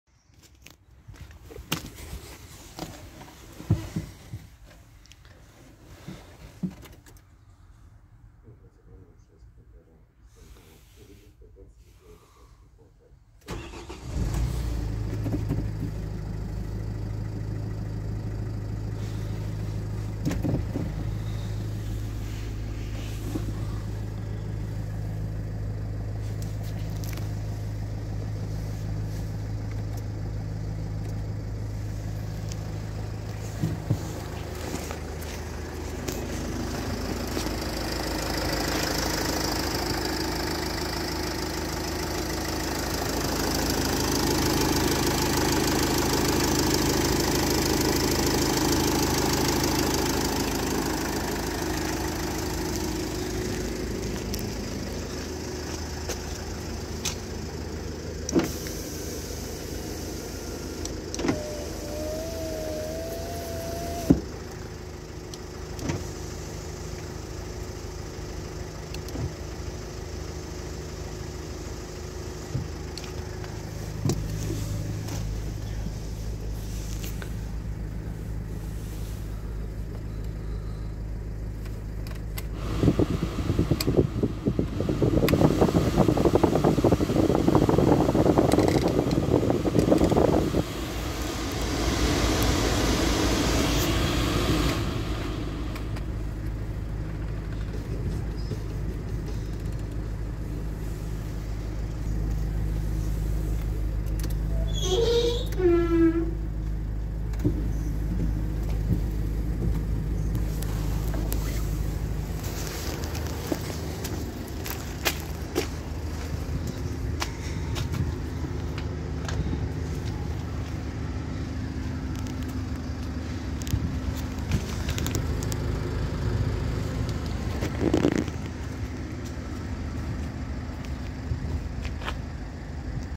Samochód do przewozu osób niepełnosprawnych OPEL Vivaro 2.0 CDTI (2008 r.) na aukcji Flotilo.pl 1z2
Flotilo.pl - licytacje maszyn i urządzeń